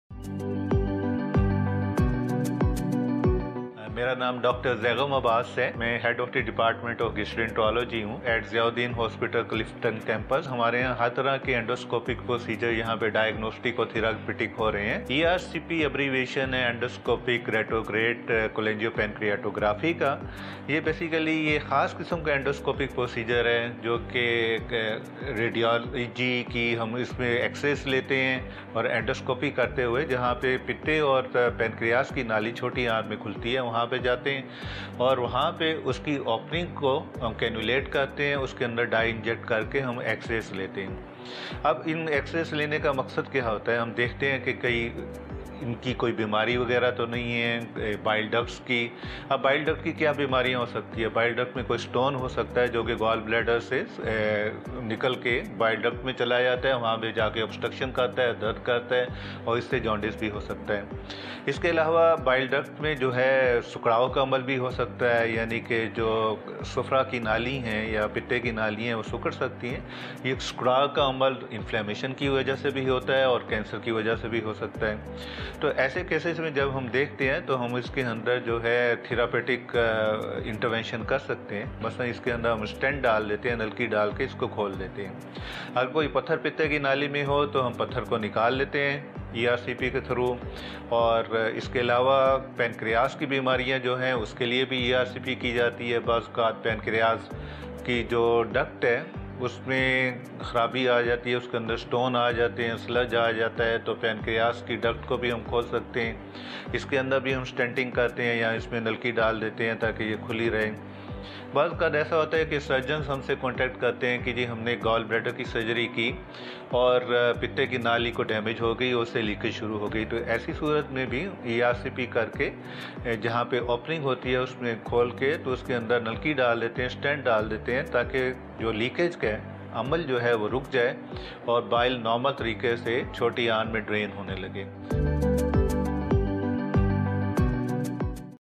Dr. Ziauddin Hospital - ERCP
Dr. Zaigham Abbas educates us on how to maintain bile duct health and the role of ERCP.